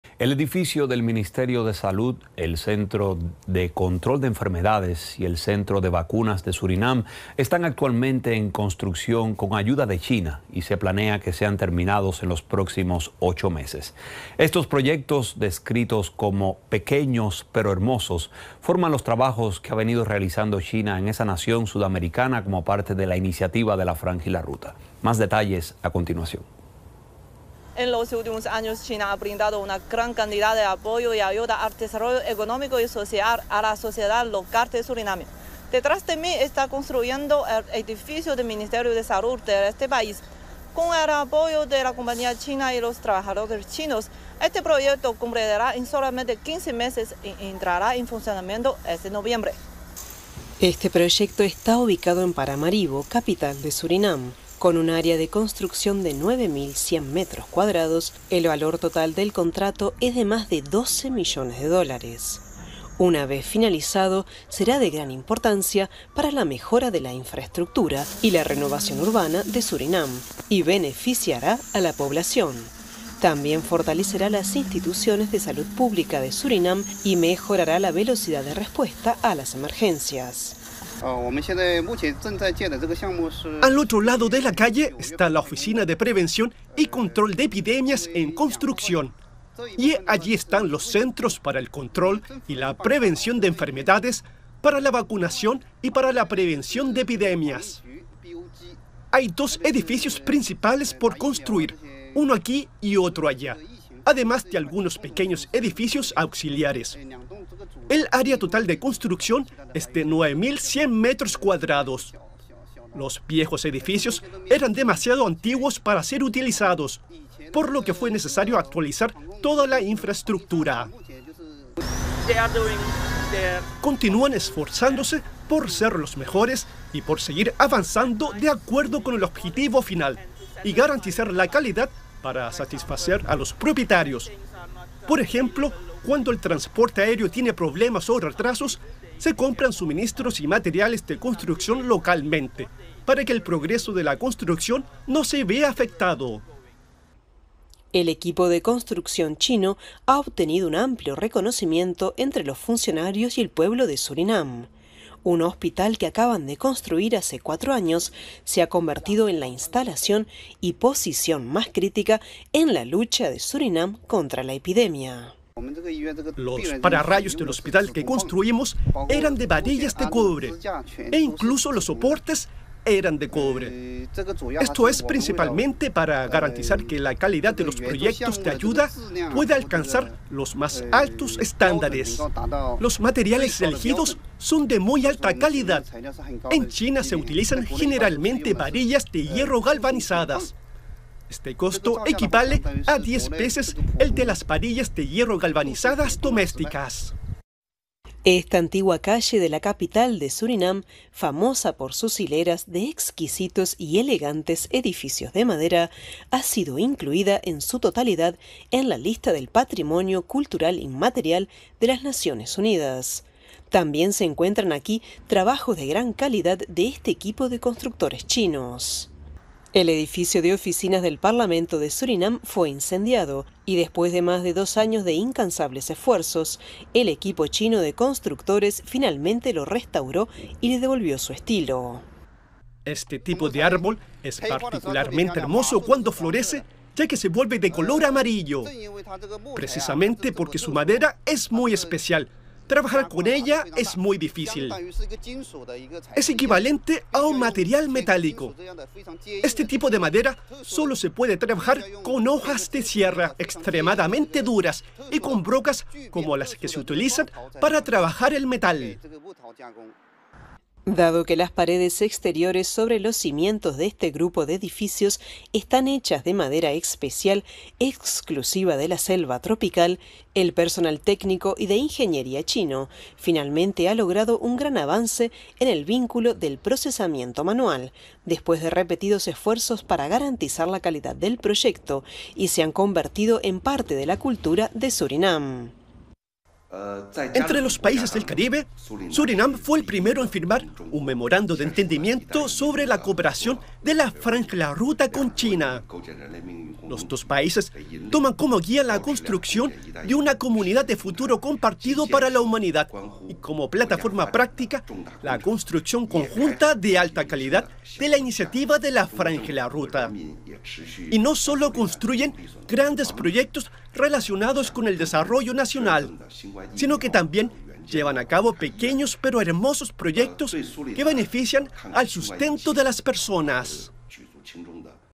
Constructores chinos crean proyectos en Surinam para mejorar calidad de las ciudades
El edificio del Ministerio de Salud, el Centro de Control de Enfermedades y el Centro de Vacunas de Surinam, están actualmente en construcción con ayuda de China, y se planea que sean terminados en los próximos ocho meses. Estos proyectos, descritos como "pequeños pero hermosos", forman los trabajos que ha venido realizando China en este país sudamericano como parte de la iniciativa de la Franja y la Ruta. Más detalles a continuación.